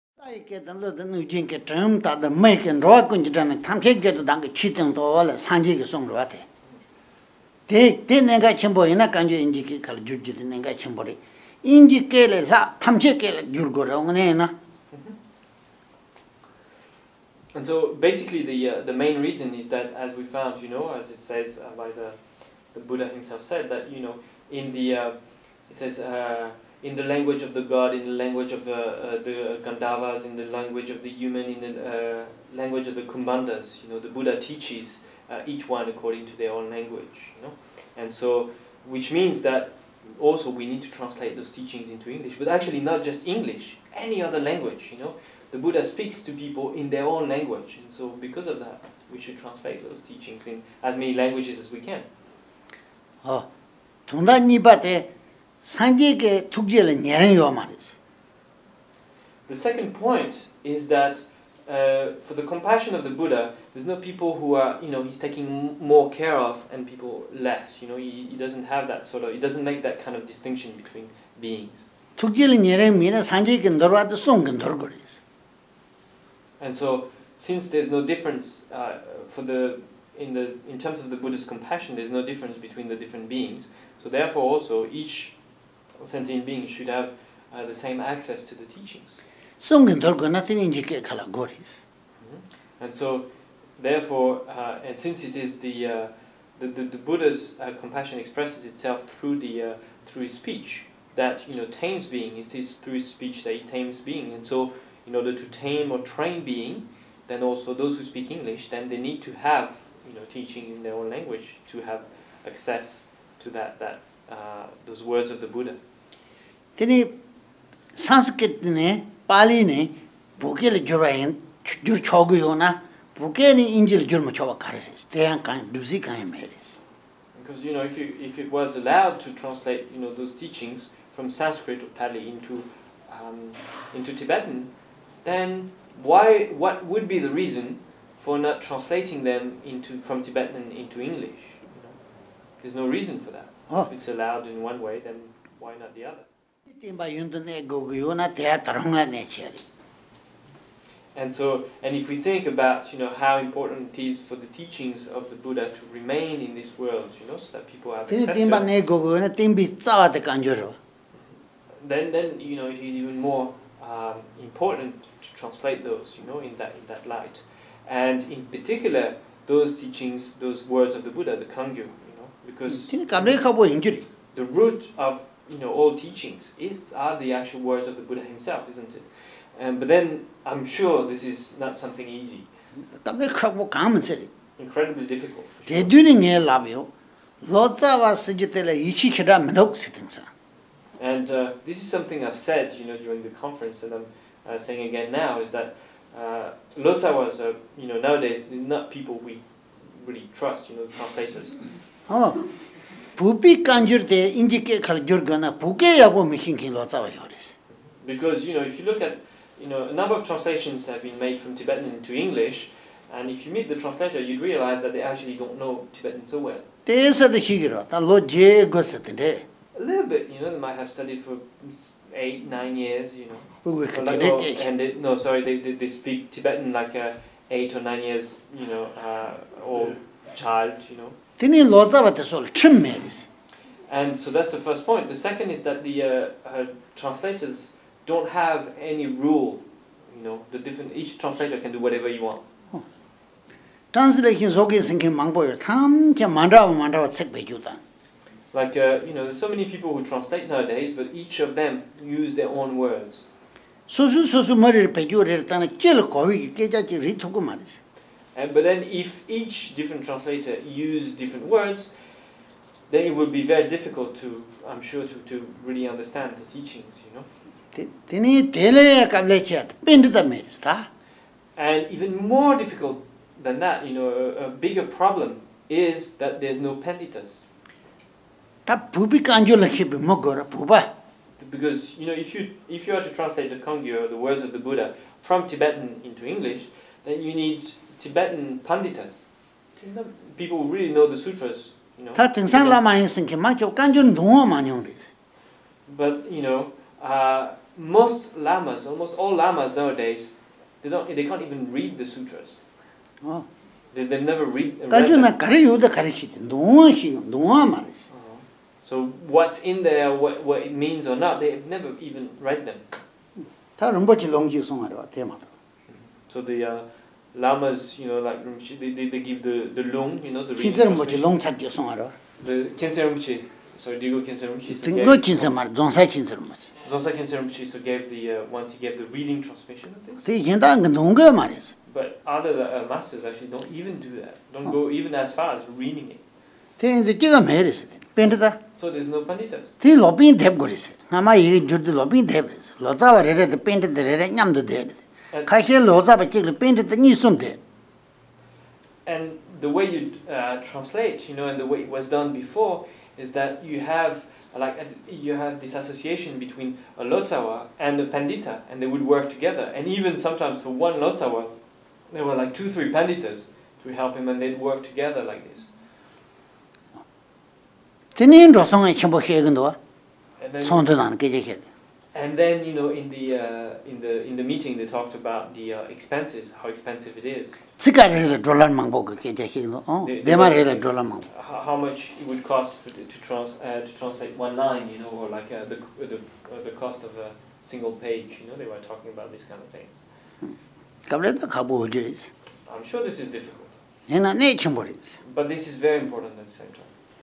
On 84000 | Orgyen Tobgyal Rinpoche, 2011
Orgyen Tobgyal Rinpoche speaks about 84000: Translating the Words of the Buddha.